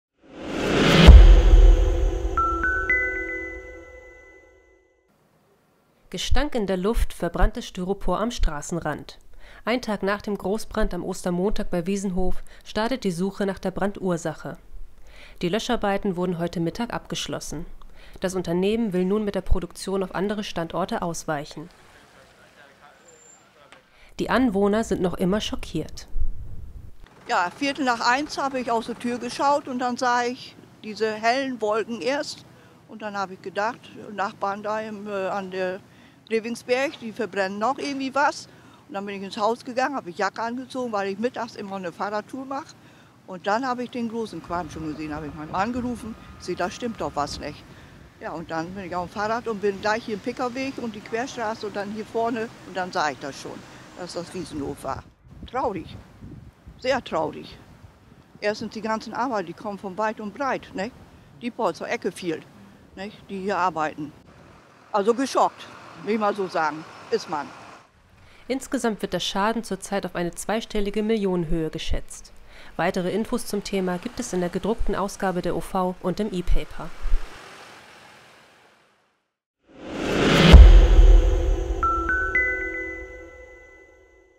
Wiesenhof am Tag nach Großbrand
Ein Tag nach dem Großbrand am Ostermontag bei Wiesenhof ist die Brandursache noch immer unklar. Während der Nacht waren noch 150 Kameraden im Einsatz, bis die Löscharbeiten heute Mittag eingestellt werden konnten. Weitere Infos zum Thema gibt es in der morgigen Ausgabe der OV und im epaper.